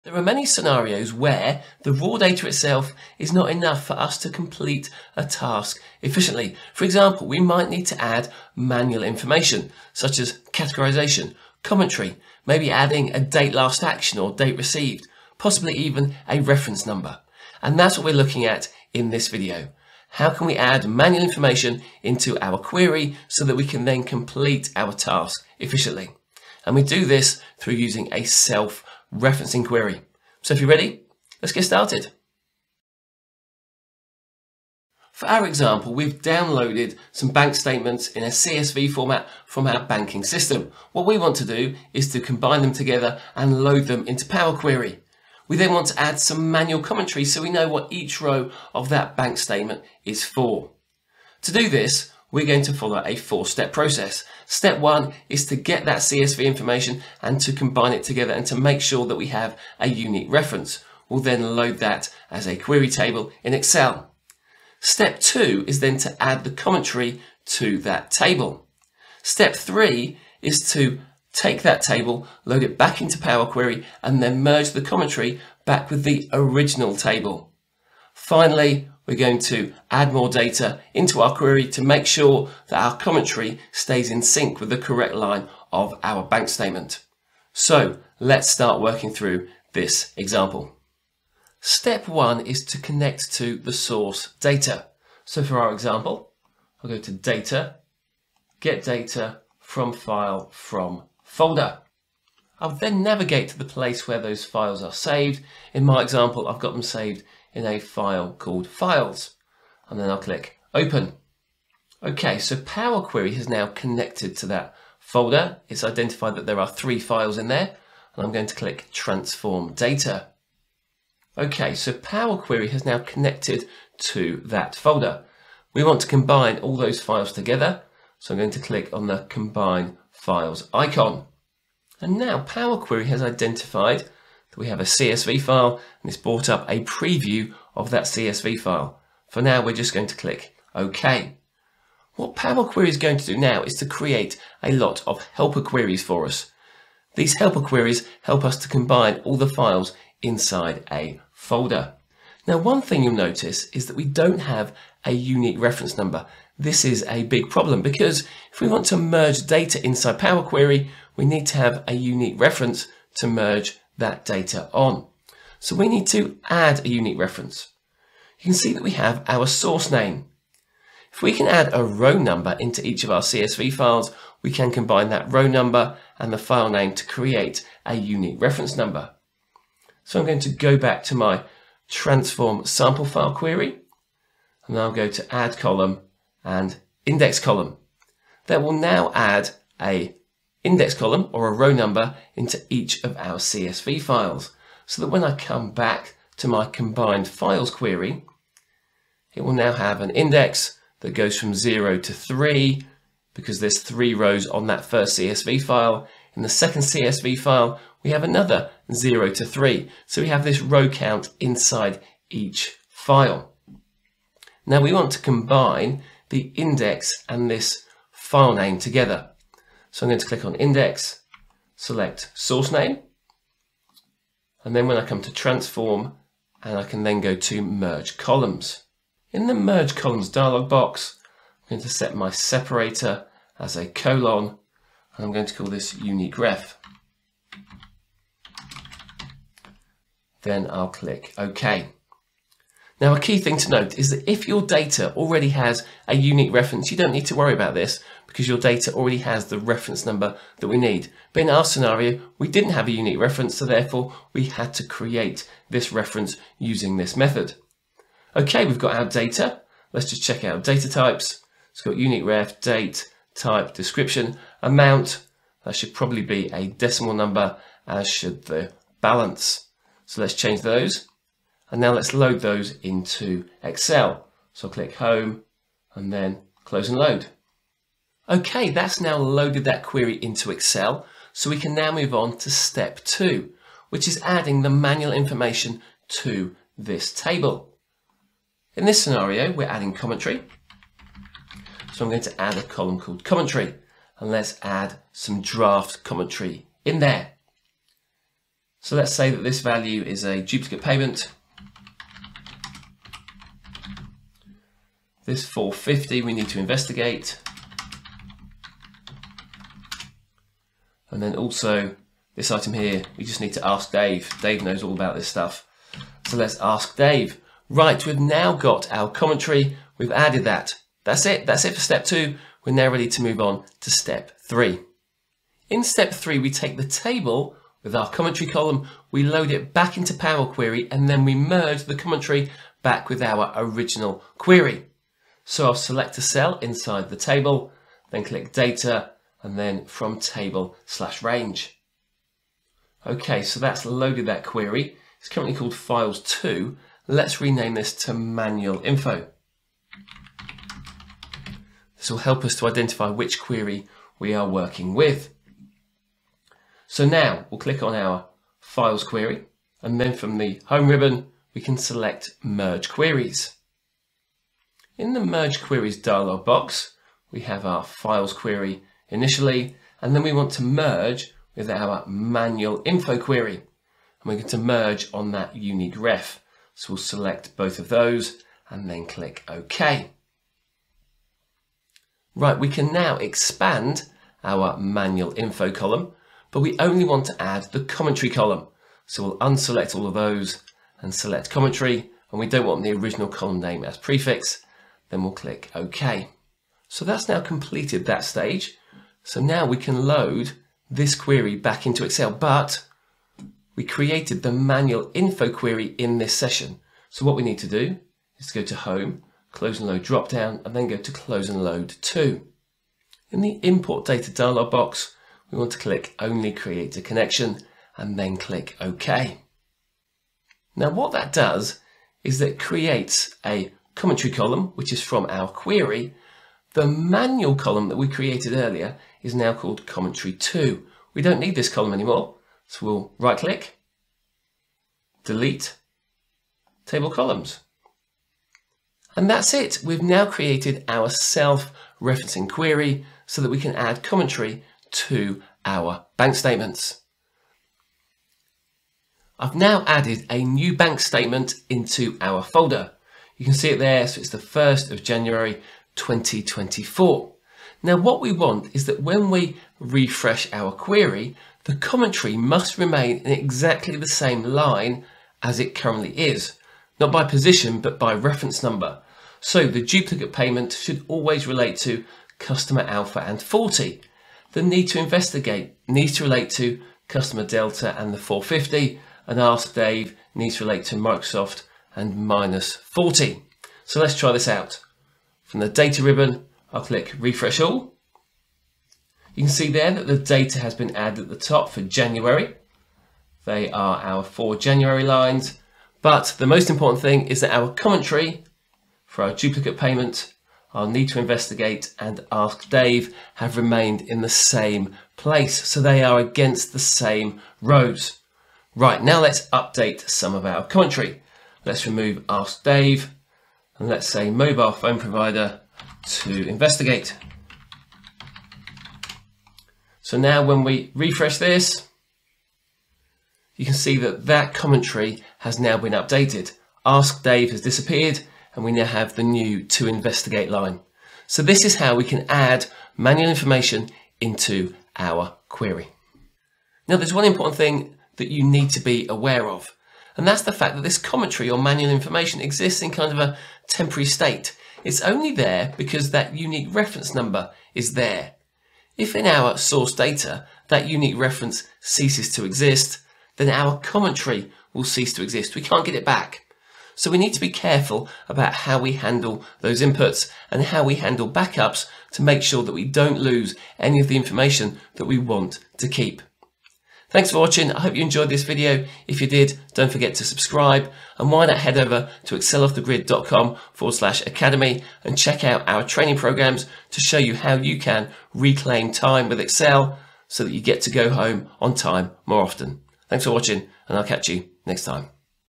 Add manual information into a query | Power Query | Excel Off The Grid
There are many scenarios where raw data itself isn't enough to complete a task efficiently. For example, we may need to add manual information, such as categorization, commentary, or reference numbers.

In this video, we look at this process and understand how to add manual information into a query.

0:00 Introduction
0:36 Example and process
1:31 Step 1: Get data & unique reference
4:43 Step 2: Add manual information
5:24 Step 3: Merge manual info with the original query
7:26 Step 4: Refreshing the data and testing
8:56 Important information
9:27 Conclusion